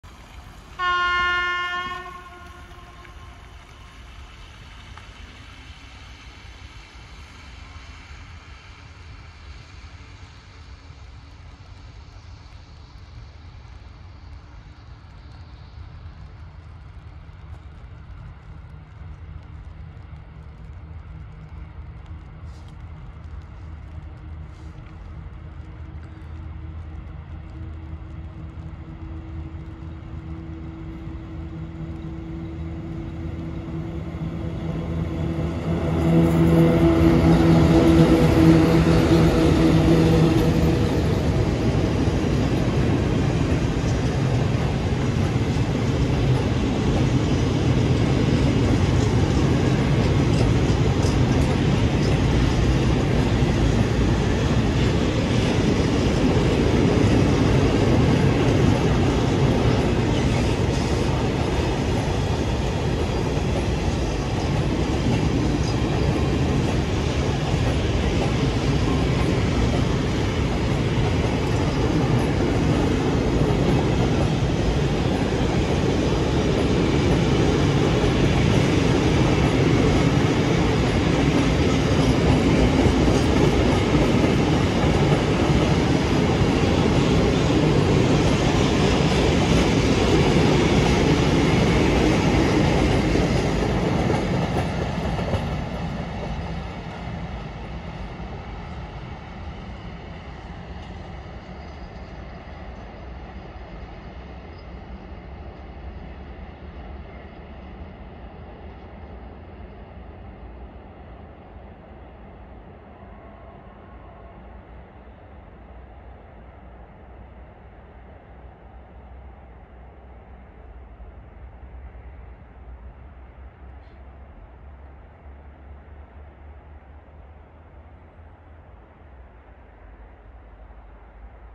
ET22-946 (PKP Cargo) + Eaos | Połaniec [4K 60]
Kolejny klasyk spod nastawni „Pł” - byk 946 rusza z próżnymi wagonami Eaos.